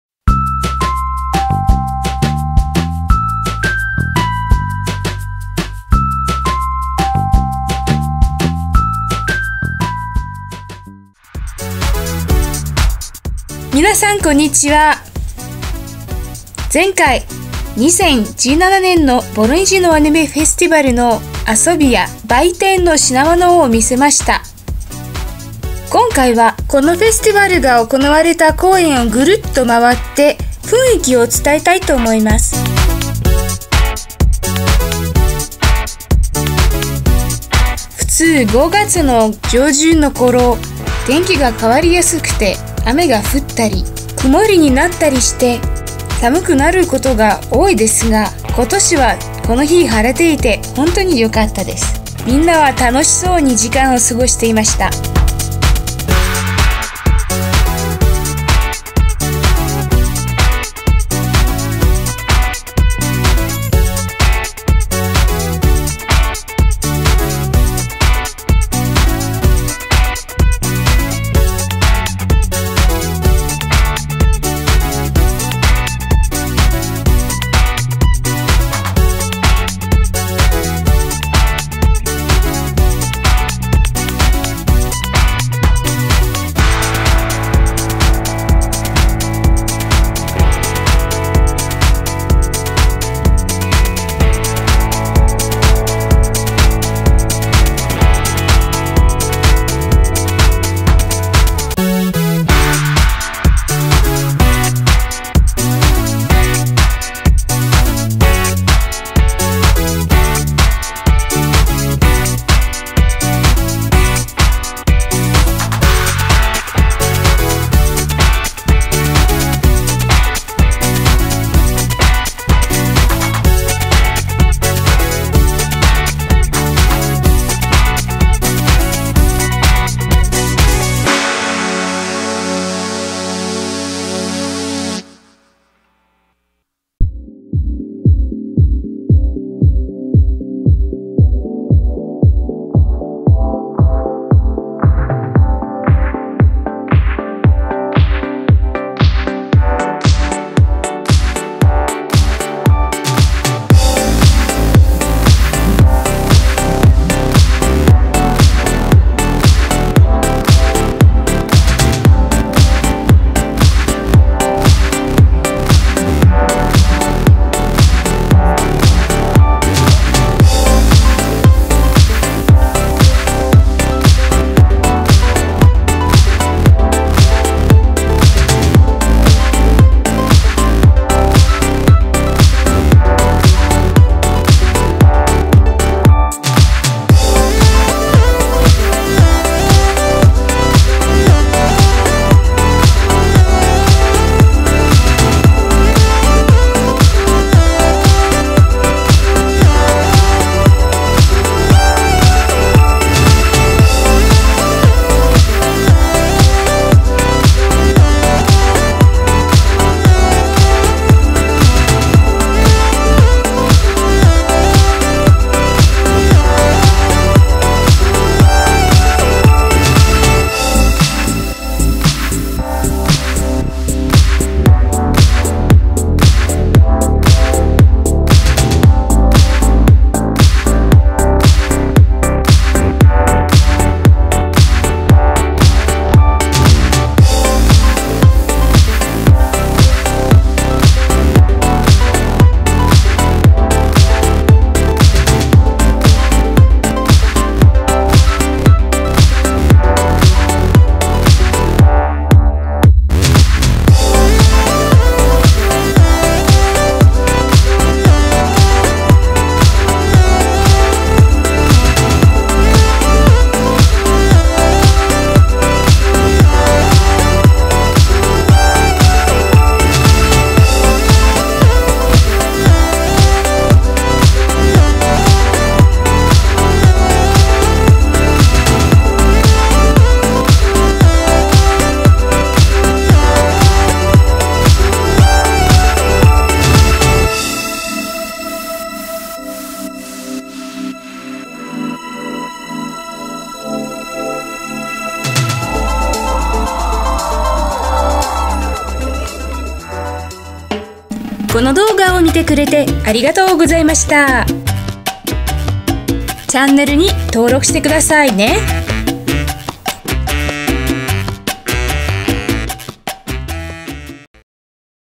アニメフェスティバル２０１７（パート２）☆公園を歩きながら楽しみましょう
ボロネジアニメフェスティバル２０１７についての動画のパート２です。今回はフェスティバルが行われた公園を回っていきましょう。途中で様々なコスプレを着ている人を見かけたり、コンサートに出席したりします。ステキな春の晴れた日を思いっきり楽しみましょう＾＾♪